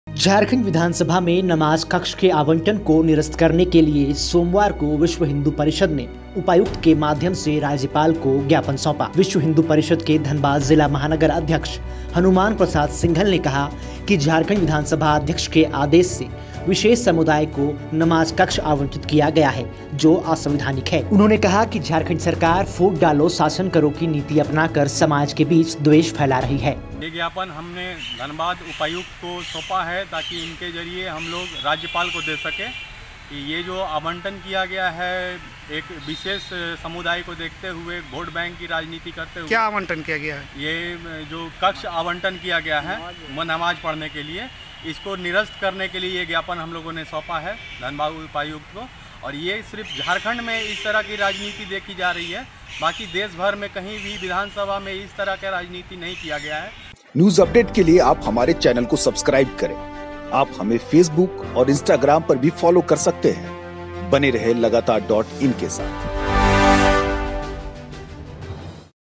Dhanbad - विश्व हिंदू परिषद ने कहा - झारखंड सरकार "फूट डालो, राज करो” की नीति अपना रही है
Dhanbad - विश्व हिंदू परिषद ने कहा -  झारखंड सरकार "फूट डालो, राज करो” की नीति अपना रही है || Live Lagatar ||